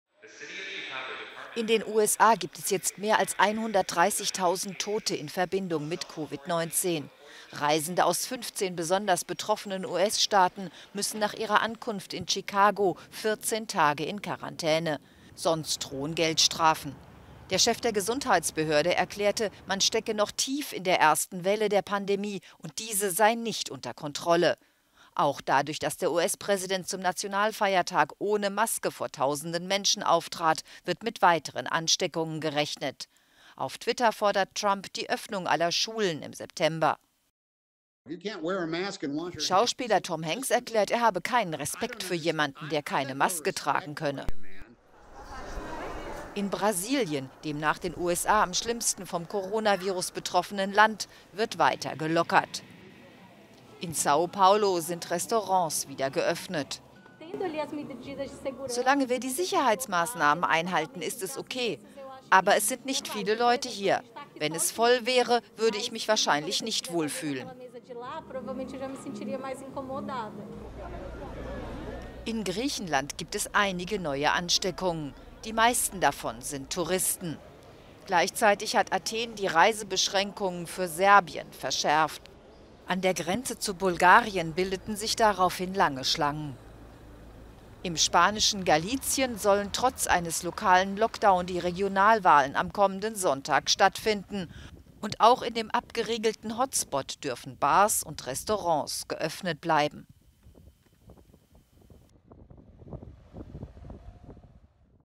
Covid-19 in USA "außer Kontrolle"
Mehr als 130.000 Menschen sind in den USA in Verbindung mit dem Coronavirus gestorben. Griechenland verhängt Reisebeschränkungen für Nachbarland Serbien.